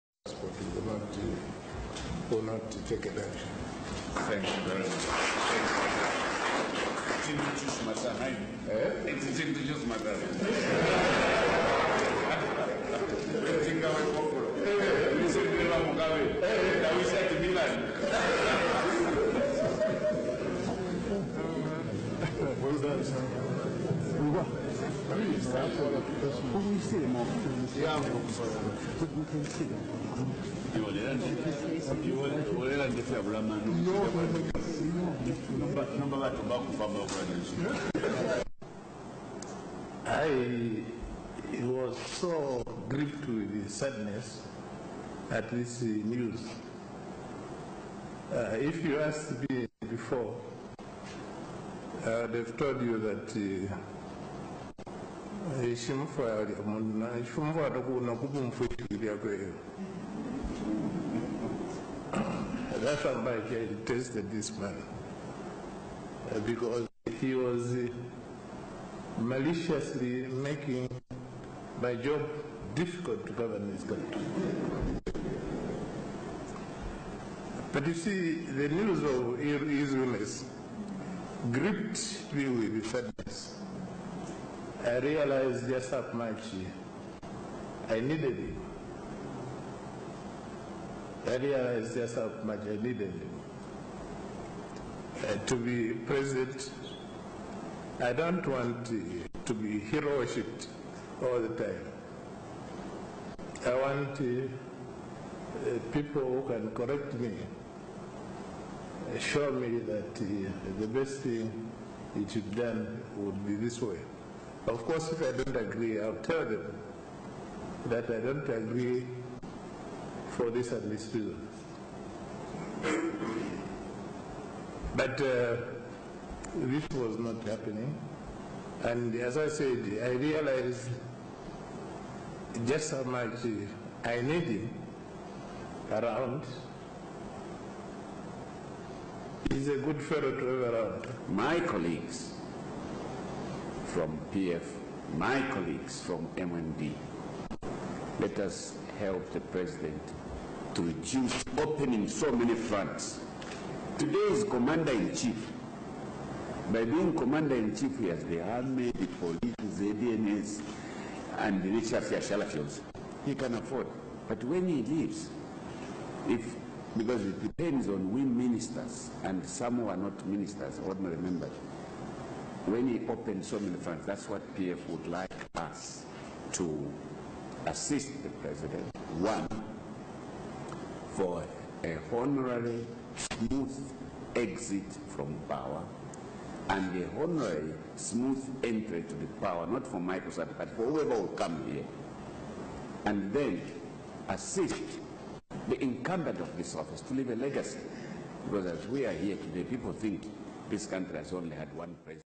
This is what Edgar Lungu and HH Would Have done (WATCH MICHAEL SATA AND MWANAWASA RECONCILIATION)
ZAMBIAN POLITICS ARE DIFFERENT FROM OTHER COUNTRIES POLITICS
Edgar Lungu#HAKAINDE HICHILEMA@ ZAMBIAN POLITICS